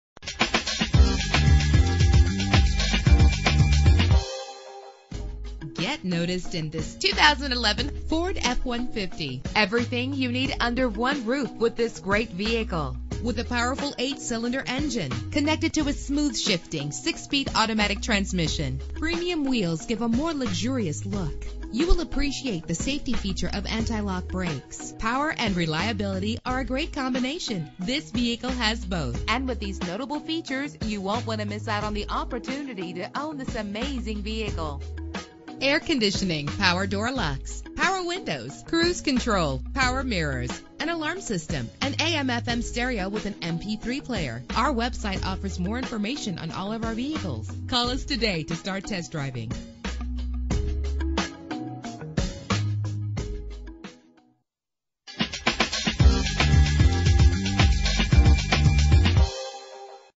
2011 Ford F-150 - Rosenberg TX
Year: 2011
Make: Ford
Model: F-150
Trim: Lariat
Engine: 6.2 liter 8 cylinder 16 valve
Transmission: 6-Speed Automatic
Color: White
Mileage: 0
Address:
27225 Southwest Fwy